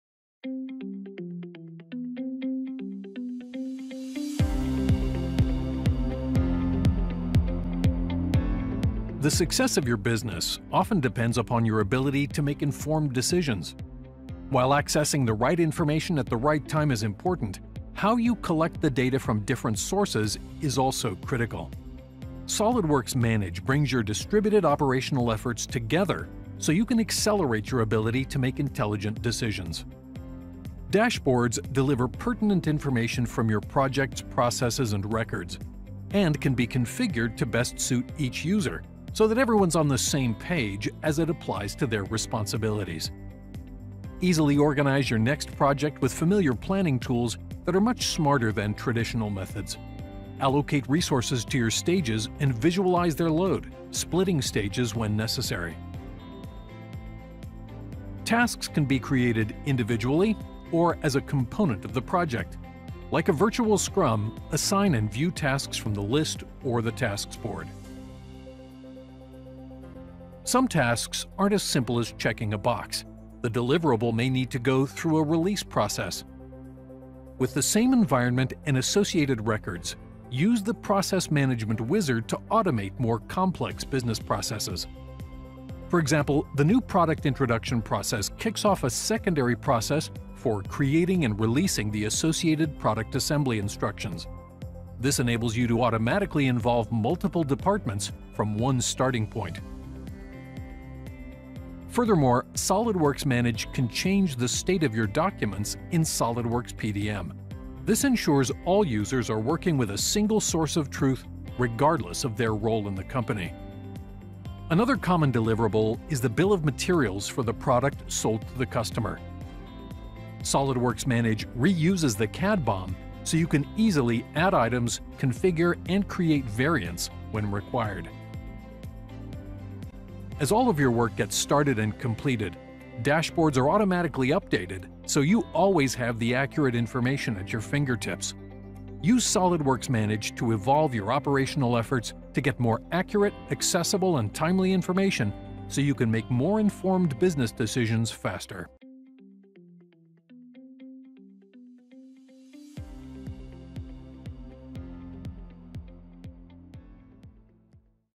SOLIDWORKS Manage 2019
SOLIDWORKS Manage 2019. SOLIDWORKS Manage delivers data from all areas of a company and provides customizable dashboards that enable you to better manage projects, processes and BOMs and make more informed decisions faster.